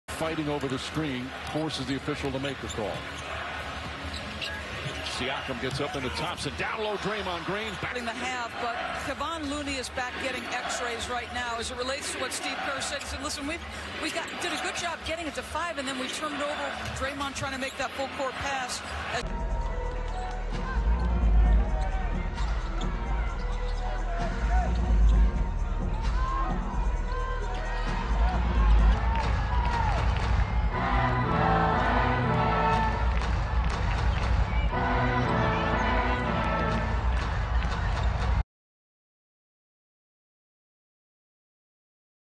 The Daily ATO: June 2, 2019
Best sets from Golden State Warriors and Toronto Raptors NBA Finals Game 2 and Las Vegas Aces vs. Connecticut Sun